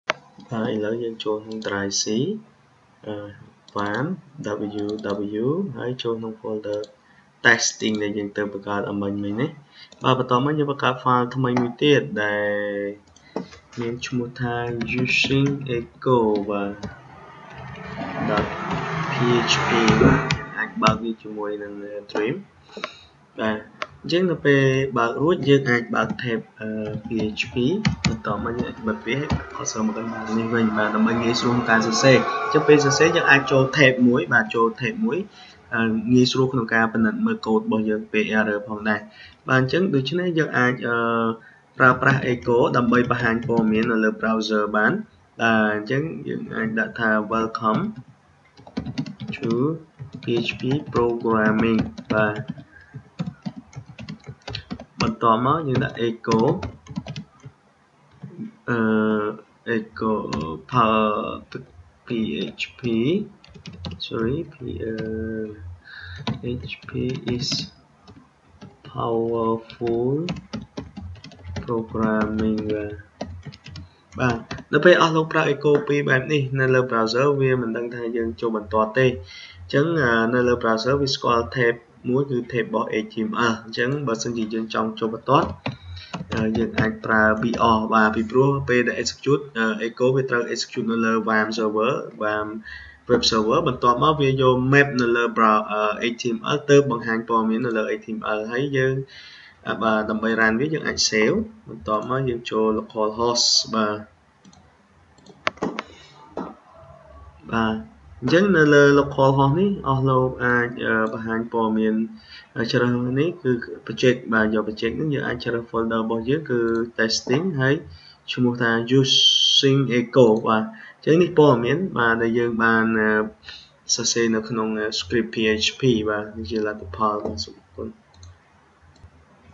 PHP Tutorial 3 Using Echo in PHP
This Video demonstrate how to using echo in PHP, PHP Tutorial